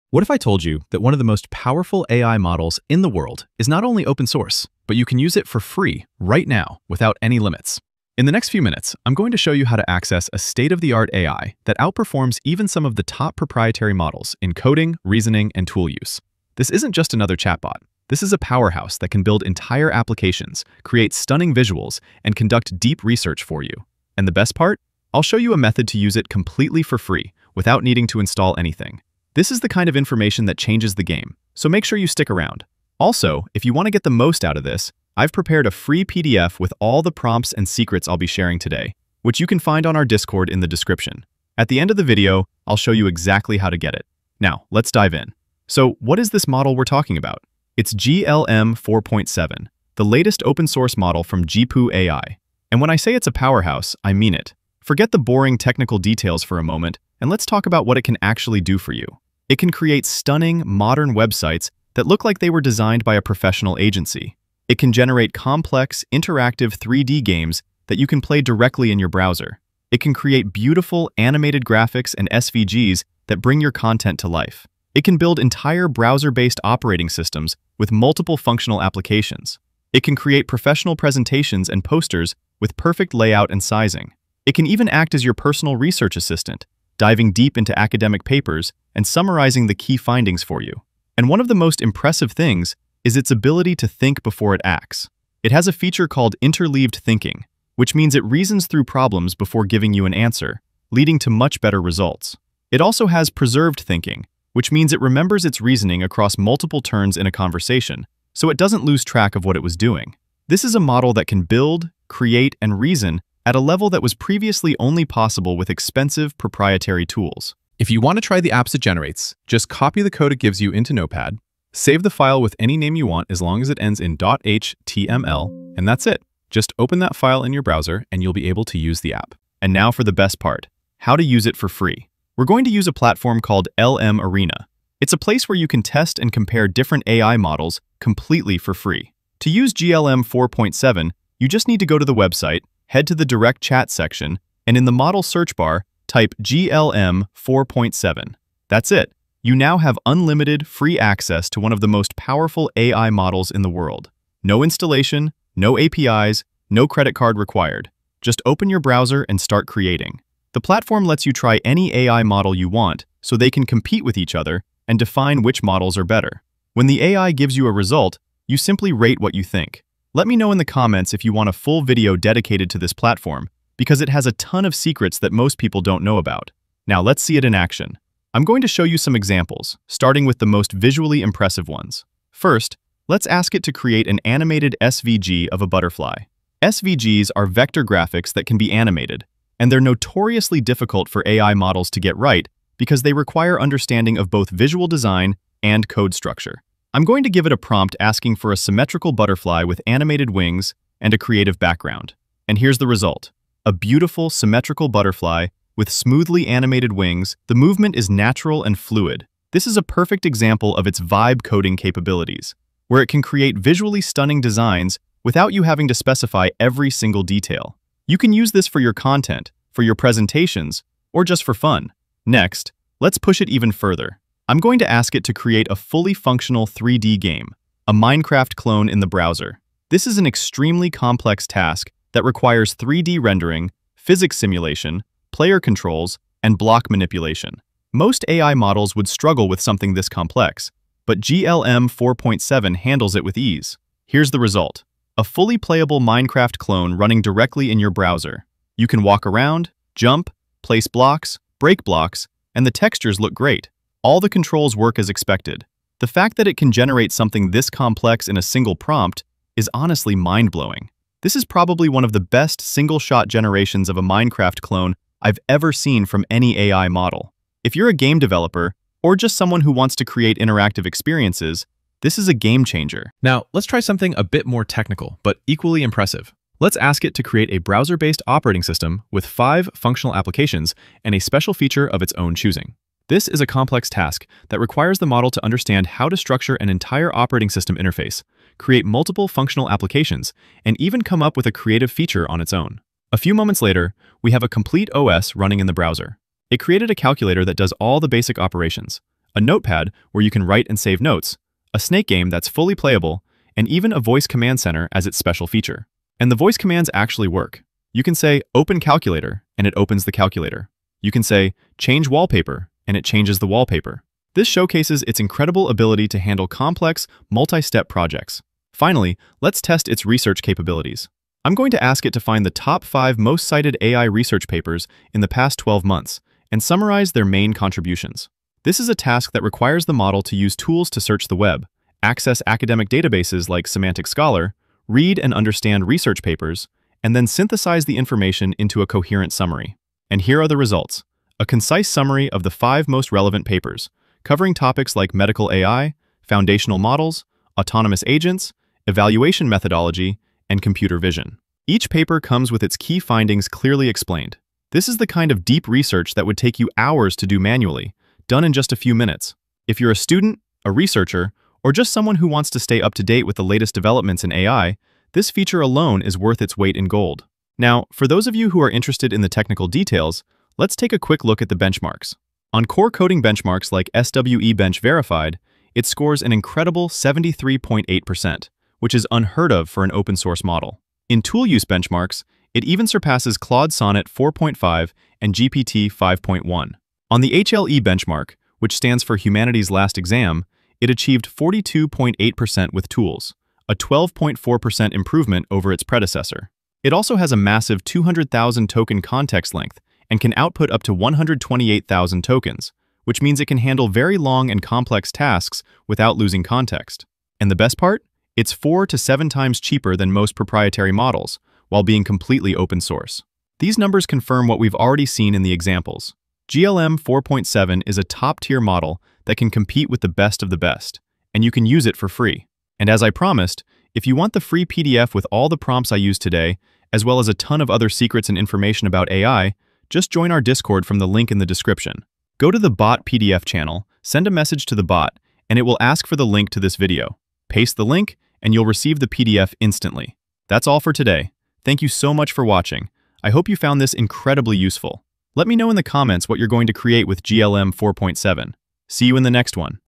Forget ChatGPT: GLM-4.7 Is FREE & Unlimited (No Install)
GLM-4.7 is one of the most powerful open-source AI models right now — and you can use it FREE in your browser with no install, no API keys, and no credit card.

In this video, I show you how to access GLM-4.7 on LM Arena and what it can actually do when you prompt it correctly:

High-level coding + reasoning + tool use
Building modern websites that look agency-made
Generating interactive apps + visuals (SVG/animations)
Deep research workflows that save hours

If you want the FREE PDF (the prompts + exact templates I used), you can grab it on our Discord here:

(Instructions to claim the PDF are inside Discord.)

“Free & unlimited” access depends on platform availability and may change over time (LM Arena can adjust limits anytime).
Always follow LM Arena rules + the model’s license/terms.
Don’t upload sensitive/private data.

✅ Chapters

00:00 The free open-source AI
00:44 What is GLM-4.7?
01:00 What it can build
01:35 Thinking features
01:59 Run apps in browser
02:11 Use it free on LM Arena
03:01 Animated SVG example
03:44 Bigger build example
04:37 Browser OS example
05:19 Deep research test
06:09 Benchmarks explained
07:03 Free PDF on Discord